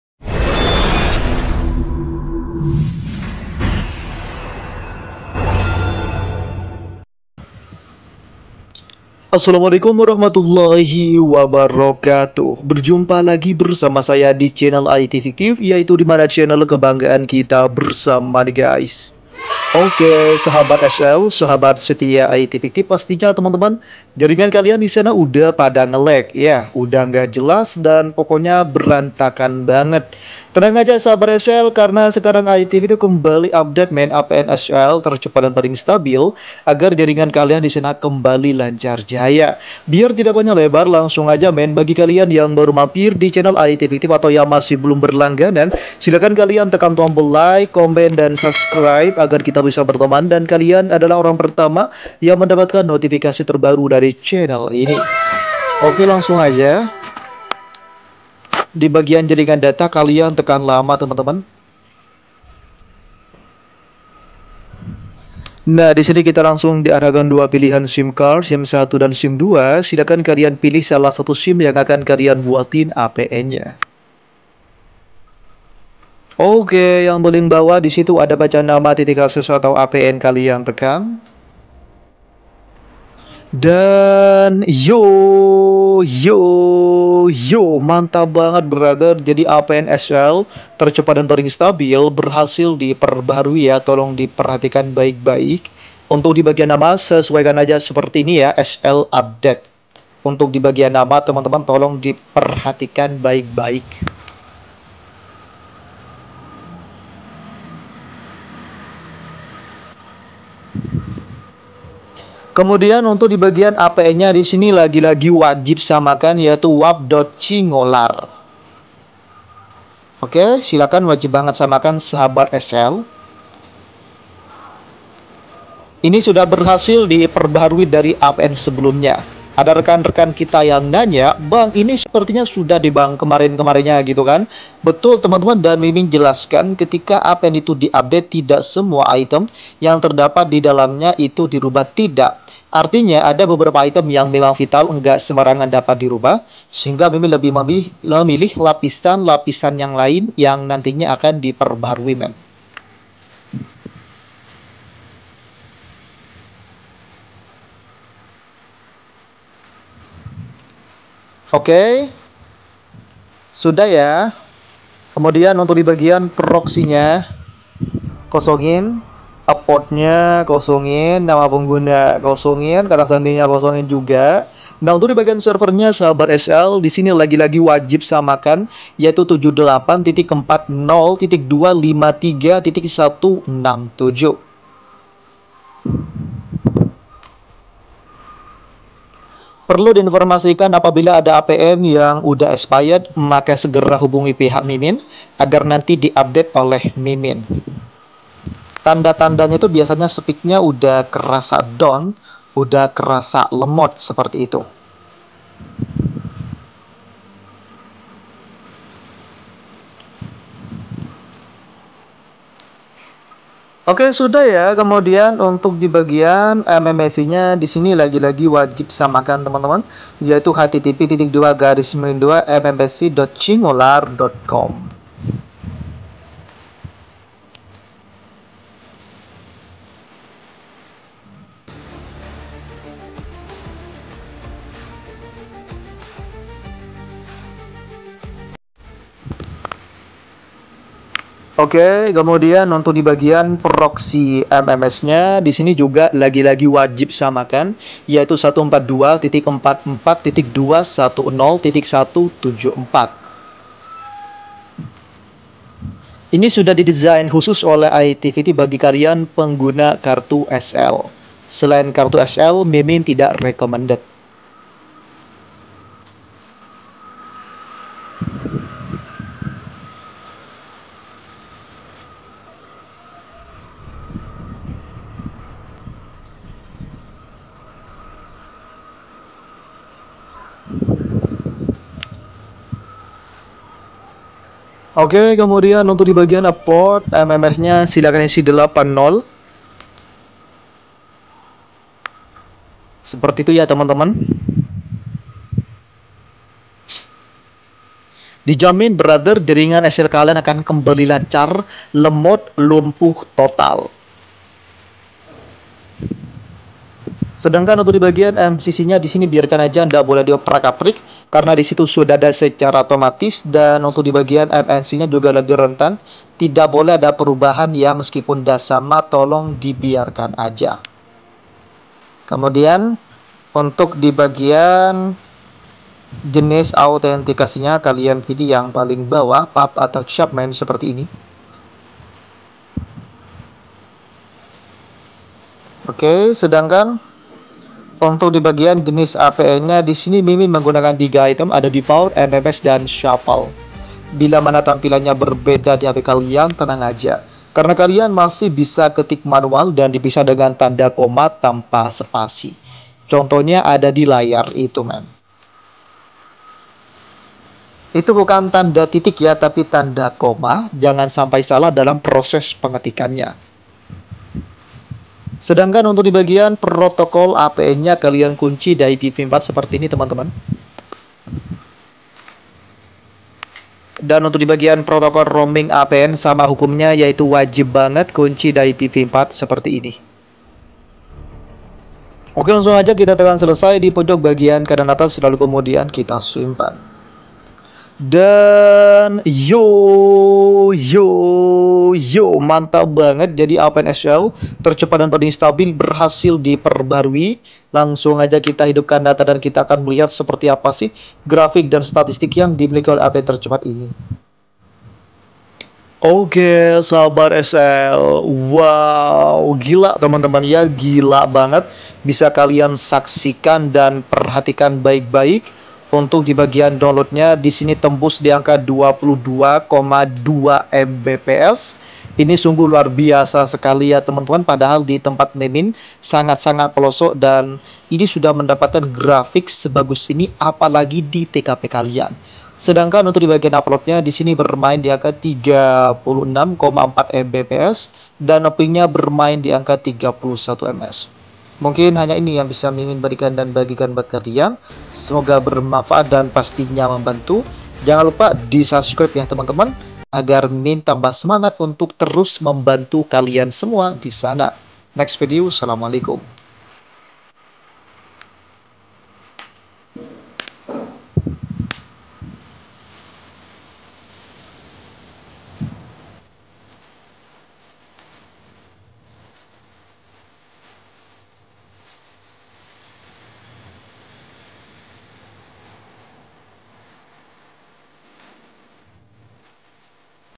Apn XL Tercepat Paling Stabil Speednya Berasa Kayak di Kantor Nasa Buruan
🔥 Vedio ini Adalah Cara Seting acces point Name Untuk Memperkuat Sinyal untuk XL Yang Lemot Setiap TKP Beda Rasa Jngan Takut Untuk Mencoba Semangat sahabat IT FIKTIF

apn game xl